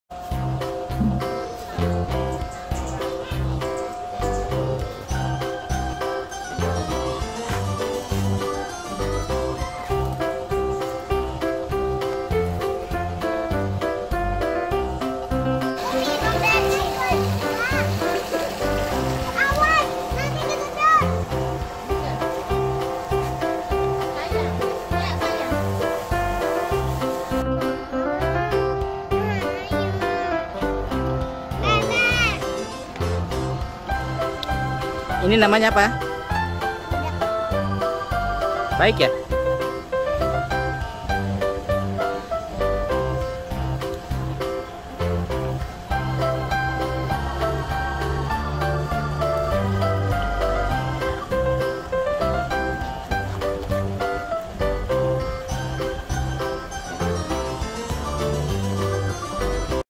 Kuda Poni Leopard Appaloosa
Bermain bersama Leopard Appaloosa atau Kuda Poni belang berbintik hitam seperti dalmatian